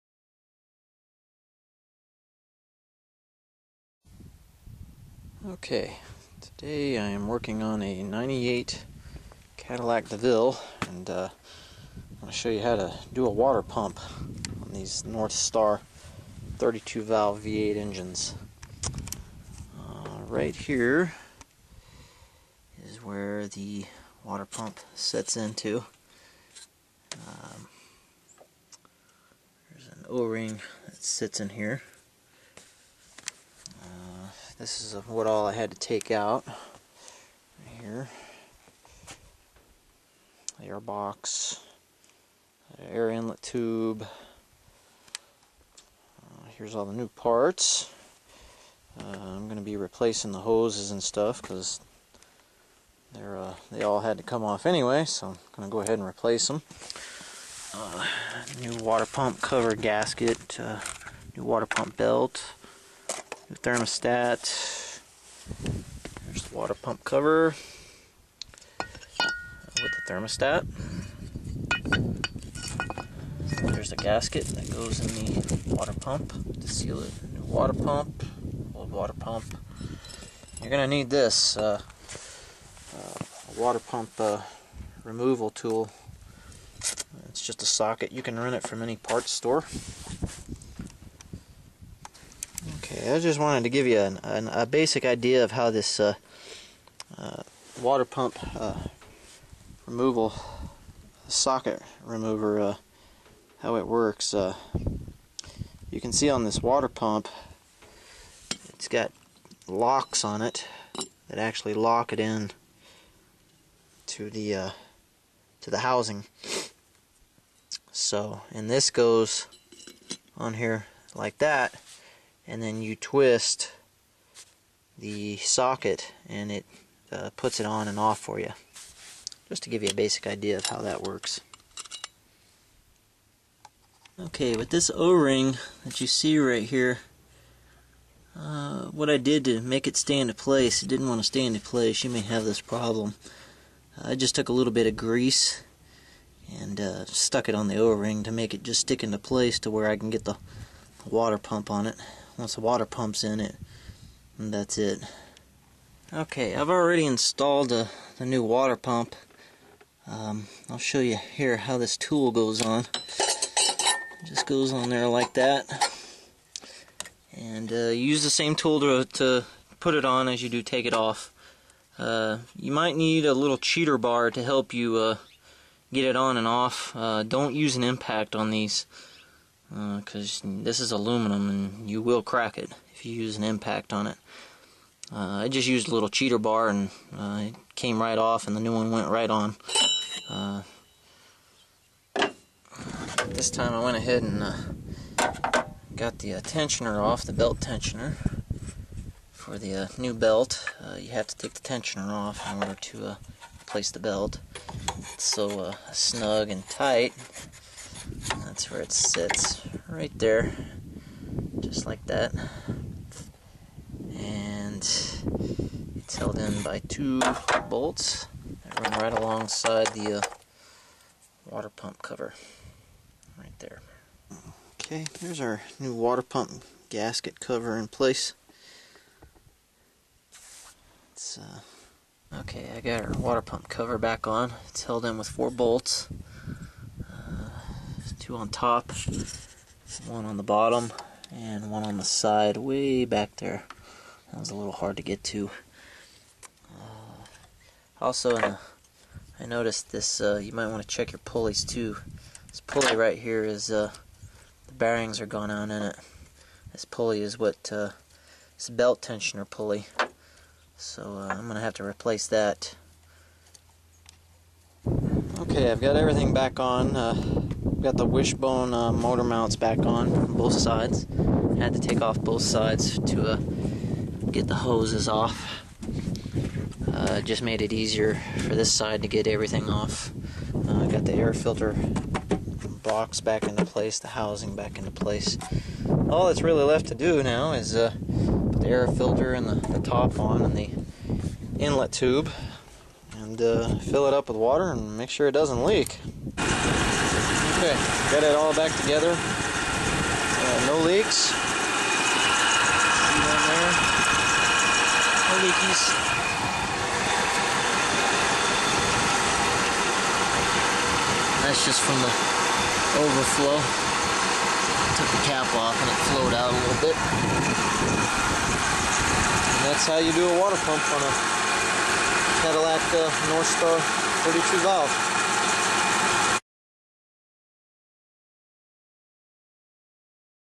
How to Install a Waterpump on a Cadillac Northstar engine.
Waterpump Installation on a Cadillac Northstar engine